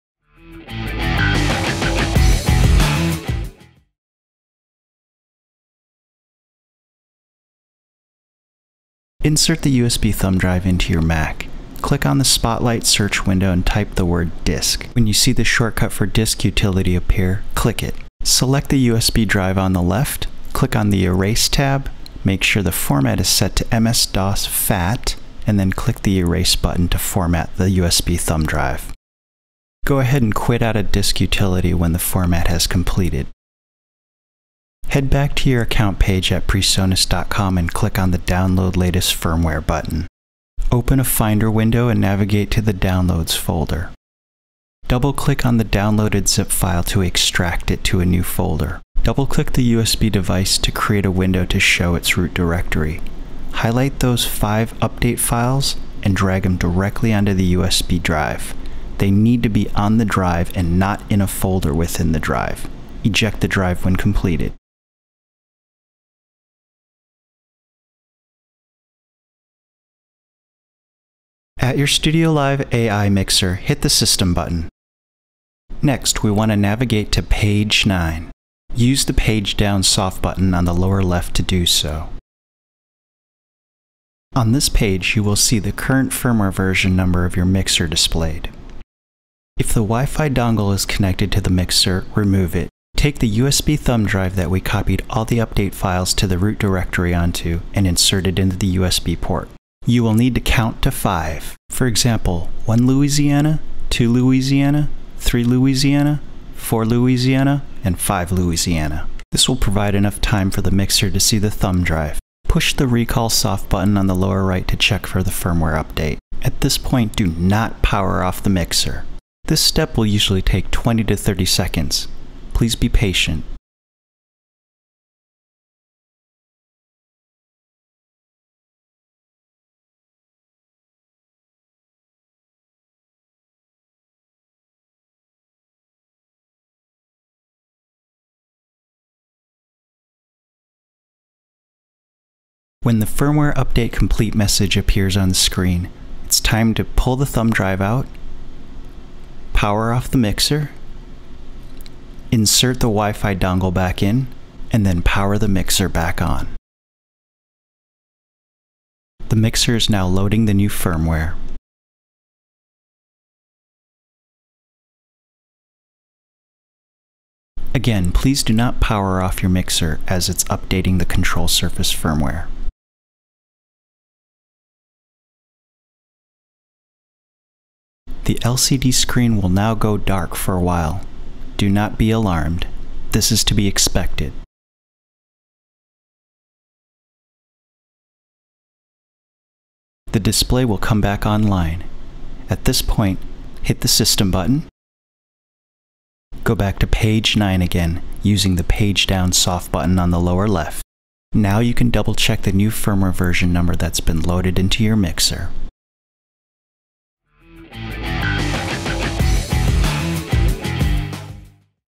How to update firmware on a PreSonus StudioLive AI Mixer (Mac OS X)
PreSonus How-To Series: updating StudioLive AI Mixer firmware for Mac OS X customers.